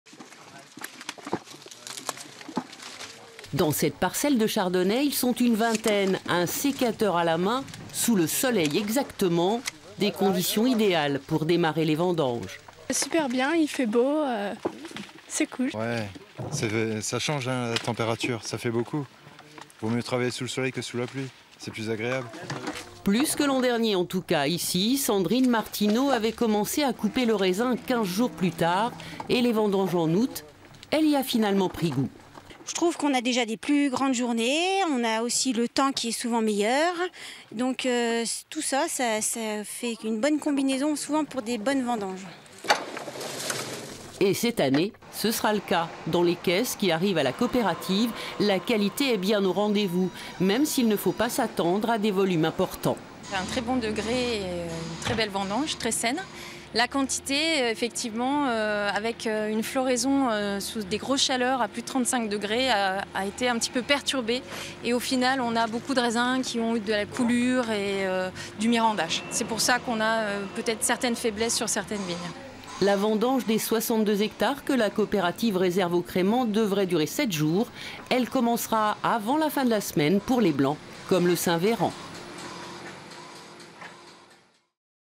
Saône-et-Loire : les vendanges ont débuté
C’est parti pour les vendanges 2017 en Bourgogne. Les premiers coups de sécateurs ont été donnés en Saône-et-Loire, où récolter au mois d’août n’a plus de caractère exceptionnel depuis quelques années déjà.

Reportage d’Anne Berger, Romy Ho-A-Chuck et Cécile Frèrebeau
Intervenants :
- Océane Roche, vendangeuse
- Sullivan Colinet, vendangeur
- Sandrine Martinot, chef d'exploitation
- Emeline Meyer-Favre, responsable du vignoble de la Coopérative des vignerons des terres secrètes